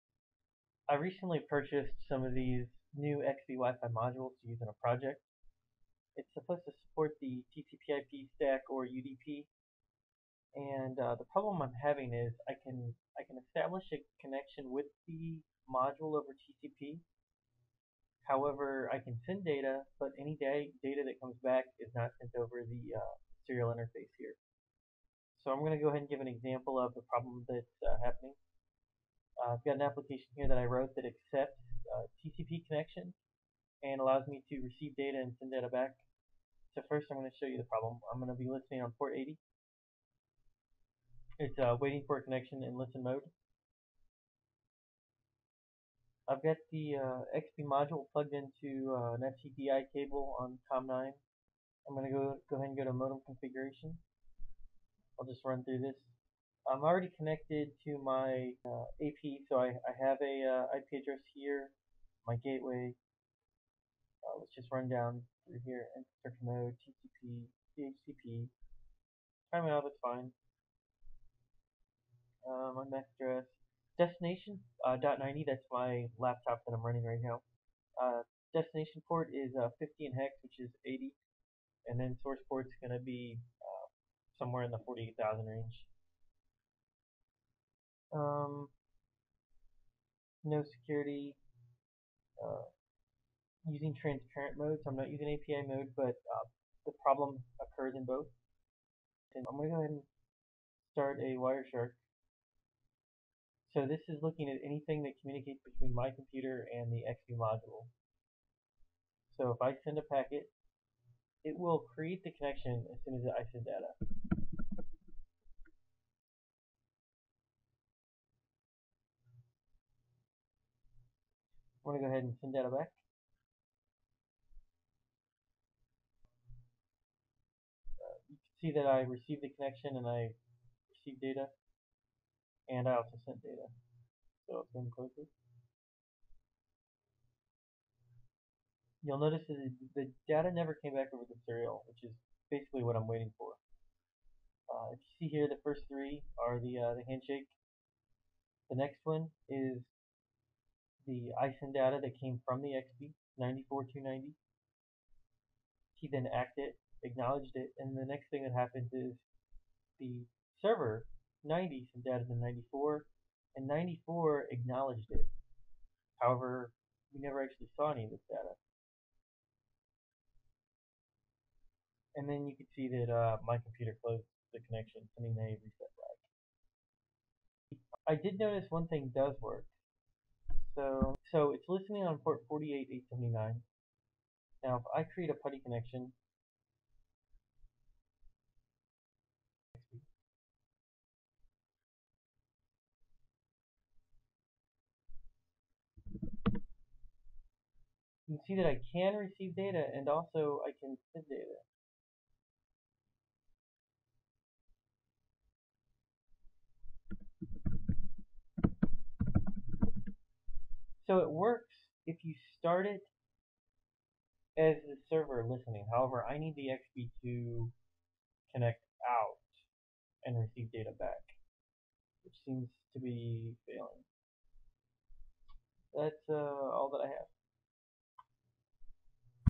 XBEE WIFI doesn't receive TCP data [FIRMWARE FIXED]
I'm using the XBEE WIFI module as a TCP/IP Client.  It correctly connects, and sends data, however, any data received by the module isn't sent over the serial UART connection.

UPDATE: (9 March 2012) - Received a personalized email from Digi's support stating that the problem has been fixed.  I tested it, and the XBEE does appear to report received TCP data over the Serial Connection now.
Modem Type = XB24-WF
Modem Firmware = 102B